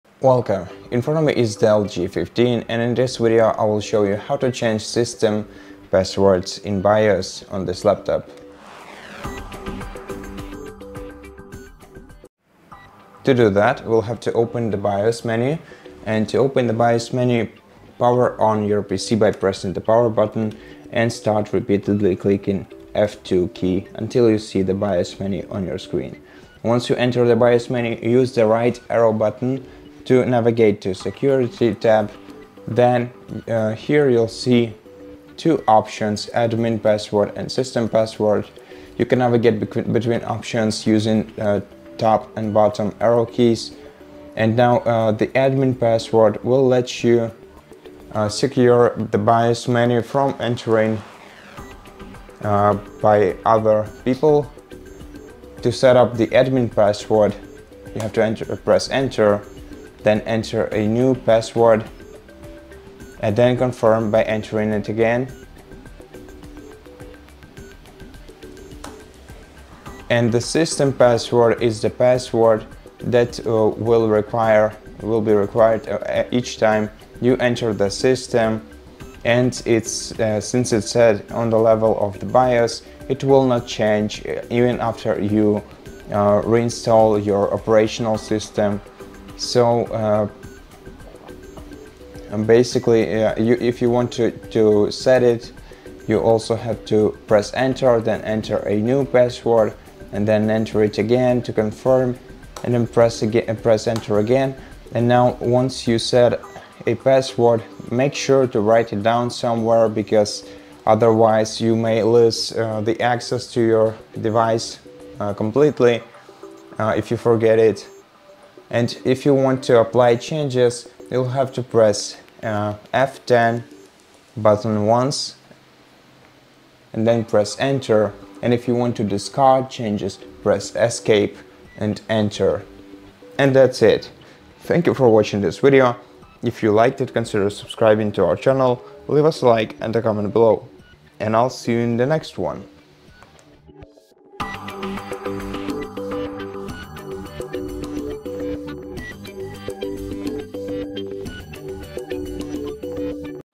DELL G15 – How to Change System Passwords in BIOS
If you want to improve the security of your DELL G15 laptop, changing the system passwords in the BIOS is an important step. In this video, you will learn how to access the BIOS menu on your DELL G15, navigate to the security tab, and set or change both the admin password and the system password. These passwords help protect your device from unauthorized access, even if the operating system is reinstalled. We’ll guide you through each step, including how to save or discard changes, and offer tips to avoid losing access to your laptop. Follow along to keep your DELL G15 safe and secure with updated BIOS passwords.

How to change system password in BIOS on DELL G15?
How to set or reset admin password in DELL G15 BIOS?
Where is the security tab in DELL G15 BIOS menu?

0:00 Introduction
0:08 Entering BIOS on DELL G15
0:26 Navigating to Security Tab
0:38 Admin and System Password Options
1:01 Setting Admin Password
1:18 Setting System Password
1:44 Important Tips for Passwords
2:05 Saving or Discarding Changes
2:13 Outro